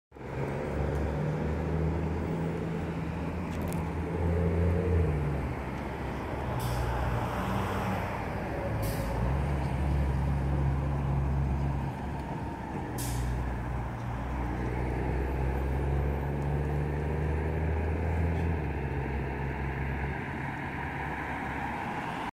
Porsche Boxster GTS Exhaust Crackles
Super car accelerating on 10/14/17. Share on Facebook, StumbleUpon, Twitter, Tumblr, YouTube, Pinterest.